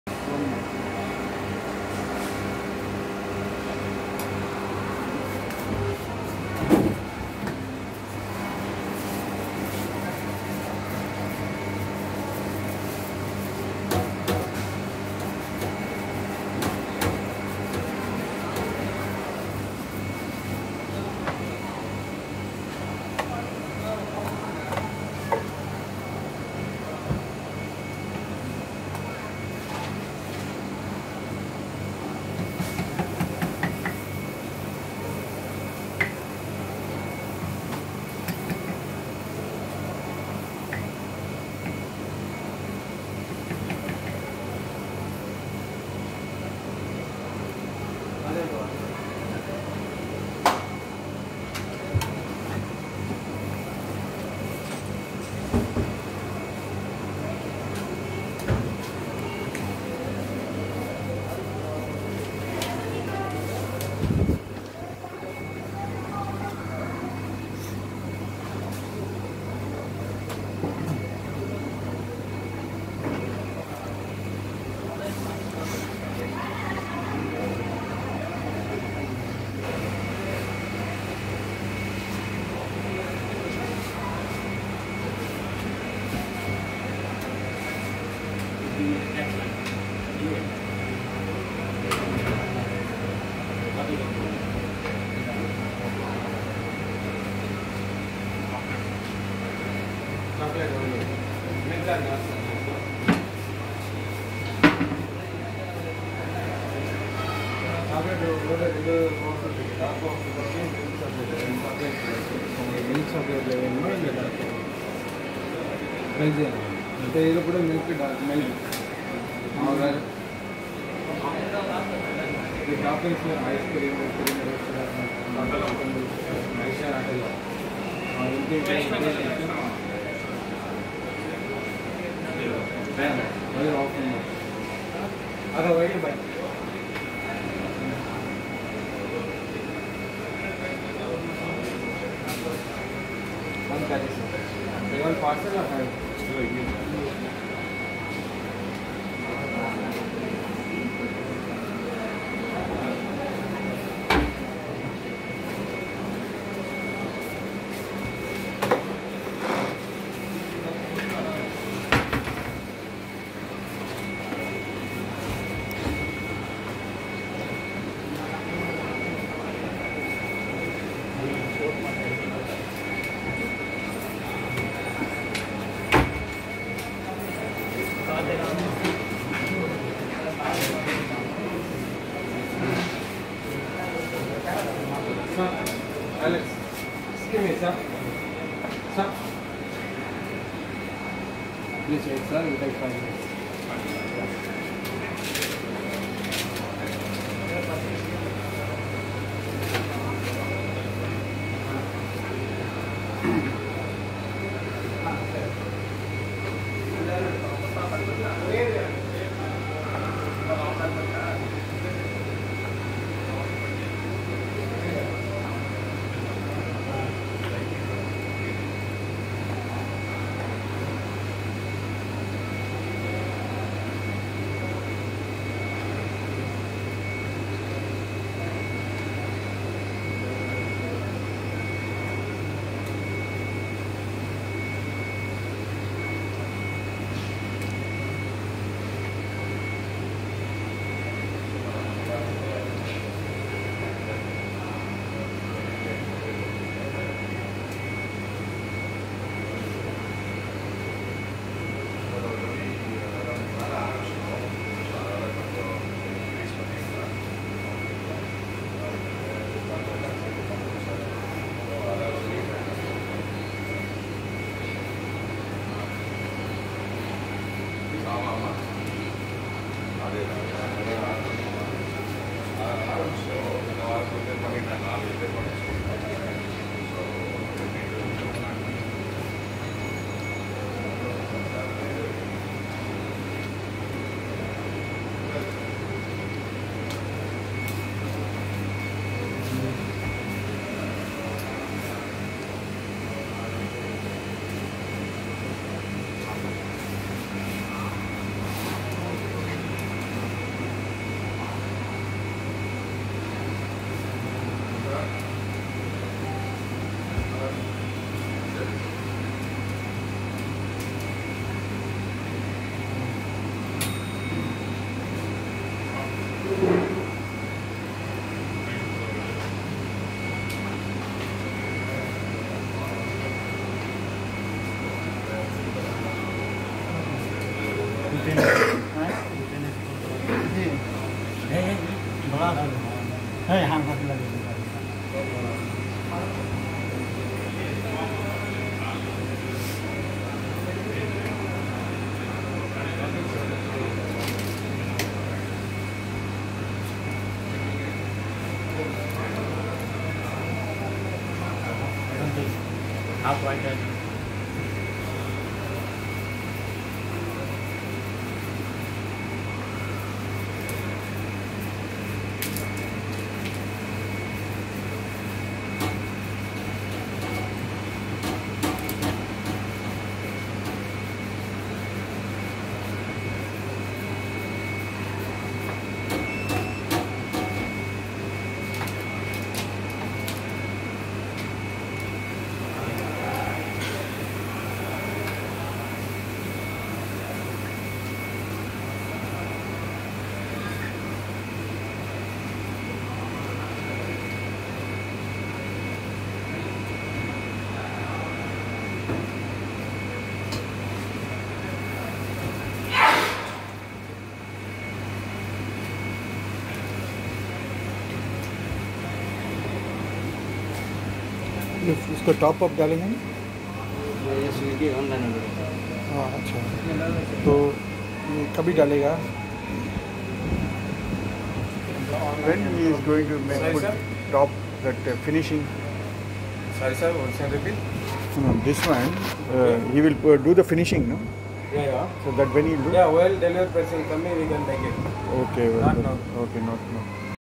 Waffle making process in the shop
Waffles making process in the Belgium waffle stores of Chennai.